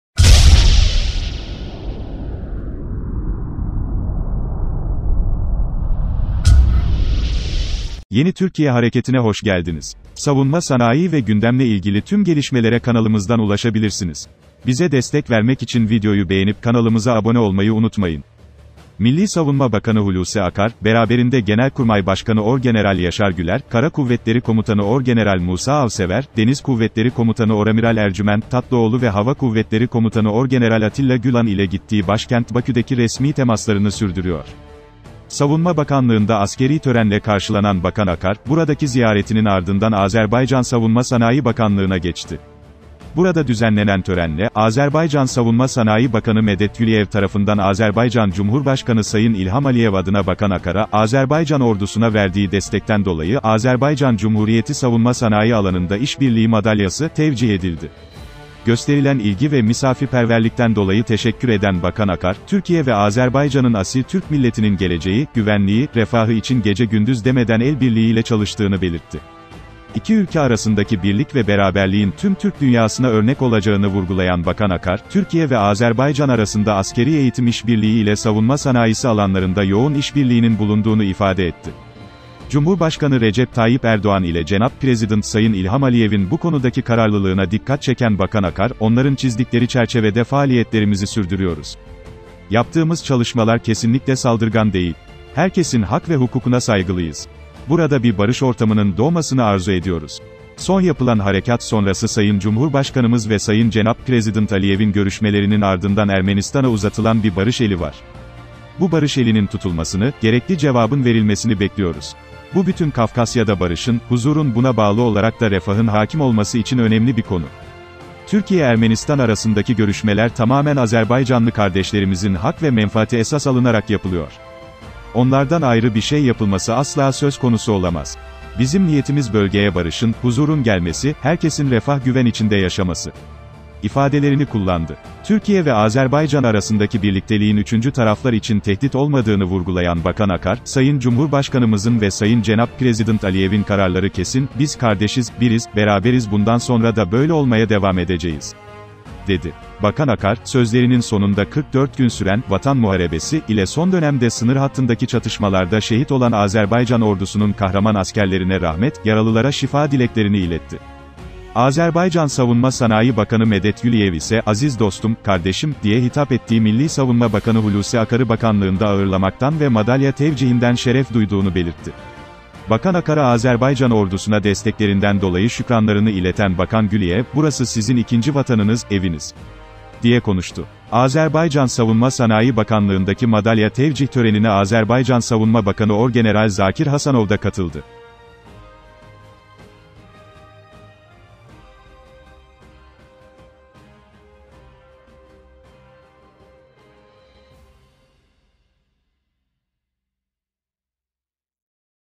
Milli Savunma Bakanı Hulusi Akar’a Azerbaycan’dan Madalya
KISA HABER METNİ :

Millî Savunma Bakanı Hulusi Akar, beraberinde Genelkurmay Başkanı Orgeneral Yaşar Güler, Kara Kuvvetleri Komutanı Orgeneral Musa Avsever, Deniz Kuvvetleri Komutanı Oramiral Ercüment Tatlıoğlu ve Hava Kuvvetleri Komutanı Orgeneral Atilla Gülan ile gittiği başkent Bakü’deki resmî temaslarını sürdürüyor.

Savunma Bakanlığında askerî törenle karşılanan Bakan Akar, buradaki ziyaretinin ardından Azerbaycan Savunma Sanayi Bakanlığına geçti. Burada düzenlenen törenle, Azerbaycan Savunma Sanayi Bakanı Medet Guliyev tarafından Azerbaycan Cumhurbaşkanı Sayın İlham Aliyev adına Bakan Akar’a, Azerbaycan ordusuna verdiği destekten dolayı “Azerbaycan Cumhuriyeti Savunma Sanayi Alanında İş Birliği Madalyası” tevcih edildi.

Gösterilen ilgi ve misafirperverlikten dolayı teşekkür eden Bakan Akar; Türkiye ve Azerbaycan’ın asil Türk milletinin geleceği, güvenliği, refahı için gece gündüz demeden el birliğiyle çalıştığını belirtti.

BİZ KİMİZ?